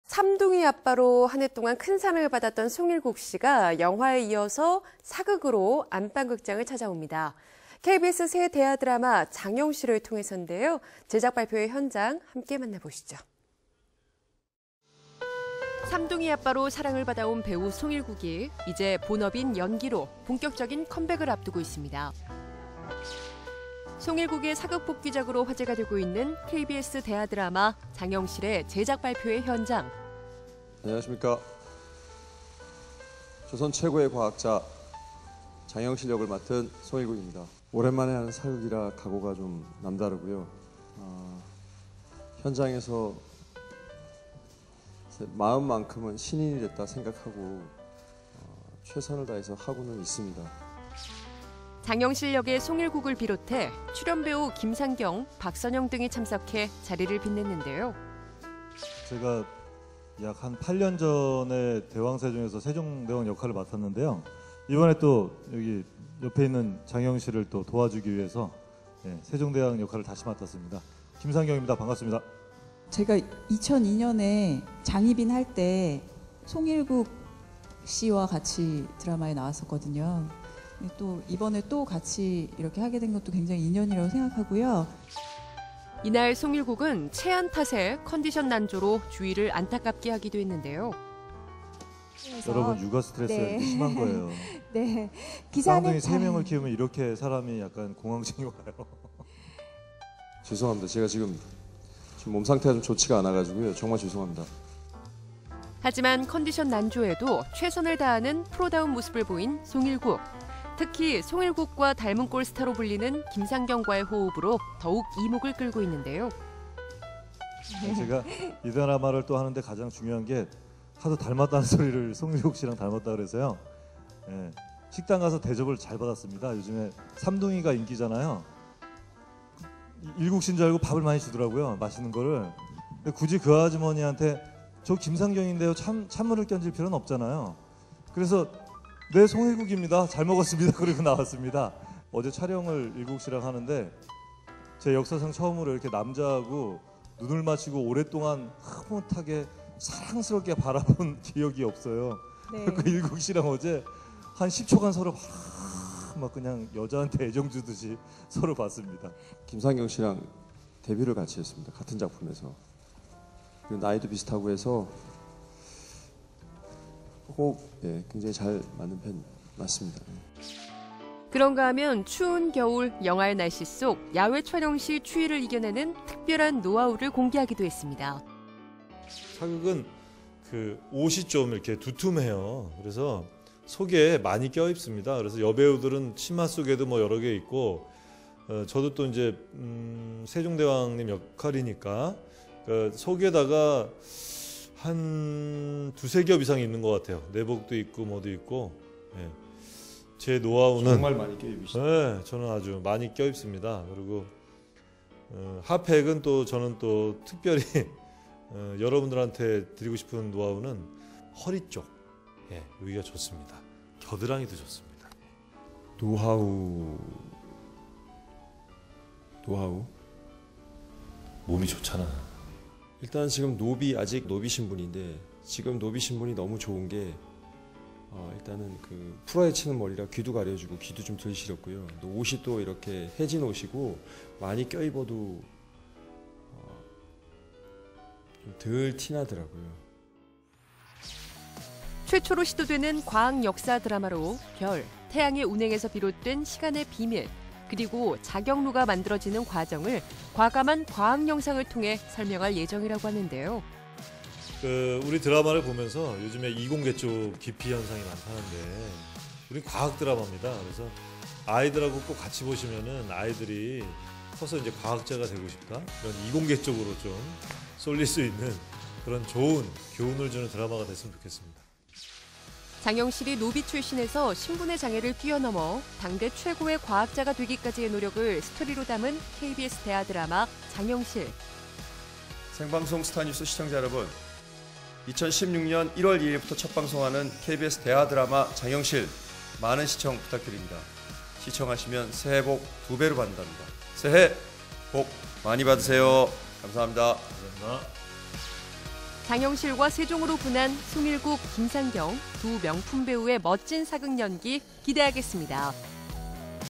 [생방송 스타 뉴스] 송일국-김상경의 사극 귀환.. 대하드라마 [장영실] 제작발표회 현장
삼둥이 아빠로 지난 한 해 큰 사랑을 받은 송일국이 영화에 이어 사극으로 안방극장을 찾아옵니다. KBS 새 대하드라마 [장영실]을 통해선데요 그 현장, 함께 만나보시죠

삼둥이 아빠로 사랑을 받아온 배우 송일국이 이제 본업인 연기로, 본격적인 컴백을 앞두고 있습니다

송일국의 사극 복귀작으로 화제가 되고 있는 KBS 대하드라마 [장영실]의 제작발표회 현장!

[현장음: 송일국]

안녕하십니까 조선 최고의 과학자 장영실 역을 맡은 송일국입니다 오랜만에 하는 사극이라 각오가 남다르고요 현장에서 마음 만큼은 신인이 됐다 생각하고 최선을 다해서 하고는 있습니다

장영실 역의 송일국을 비롯해 출연배우 김상경, 박선영 등이 참석해 자리를 빛냈는데요

[현장음: 김상경]

제가 약 8년 전에 [대왕 세종]에서 세종대왕 역할을 맡았는데요 이번에 또 옆에 있는 장영실을 도와주기 위해서 세종대왕 역할을 다시 맡았습니다 김상경입니다 반갑습니다

[현장음: 박선영]

제가 2002년에 [장희빈]할 때 송일국 씨와 같이 드라마에 나왔었거든요 또 이번에 같이 이렇게 하게 된 것도 인연이라고 생각하고요

이날 송일국은  체한 탓에 컨디션 난조로 주위를 안타깝게 하기도 했는데요

[현장음: 김상경]

여러분 육아스트레스가 이렇게 심한 거예요 쌍둥이 세명을 키우면 이렇게 사람이 약간 공황장애가 와요

[현장음: 송일국]

죄송합니다 제가 지금 몸 상태가 좋지 않아서요 정말 죄송합니다

하지만 컨디션 난조에도 최선을 다하는 프로다운 모습을 보인 송일국! 특히 송일국과 닮은꼴 스타로 불리는 김상경과의 호흡으로 더욱 이목을 끌고 있는데요

[현장음: 김상경]

제가 이 드라마를 하는데 가장 중요한 게 하도 닮았다는 소리를, 송일국 씨랑 닮았다고 그래서요 식당 가서 대접을 잘 받았습니다 요즘에 삼둥이가 인기잖아요 일국 씨인줄 알고 밥을 많이 주더라고요 맛있는 걸.. 굳이 그 아주머니에게 저 김상경인데요 찬물을 끼얹을 필요는 없잖아요 그래서 '네 송일국입니다 잘먹었습니다' 그러고 나왔습니다 어제 촬영을 일국씨랑 하는데, 제가 역사상 남자하고 눈을 맞추고 오랫동안 흐믓하게 사랑스럽게 바라본 기억이 없어요 한 10초간 서로 막~ 여자한테 애정주듯이 서로 봤습니다

[현장음: 송일국]

김상경 씨랑 데뷔를 같이 했습니다 같은 작품에서, 그리고 나이도 비슷하고 해서 호흡 잘 맞는 편이에요 맞습니다

그런가하면 추운 겨울 영하의 날씨 속, 야외 촬영시 추위를 이겨내는 특별한 노하우를 공개하기도 했는데요

[현장음: 김상경]

사극은 옷이 좀 두툼해요 그래서 속에 많이 껴입습니다 그래서 여배우들은 치마속에 여러 개 입고 저도 세종대왕님 역할이니까 속에다가 한 두세겹 이상 입는 거 같아요 내복도 입고 뭐도 입고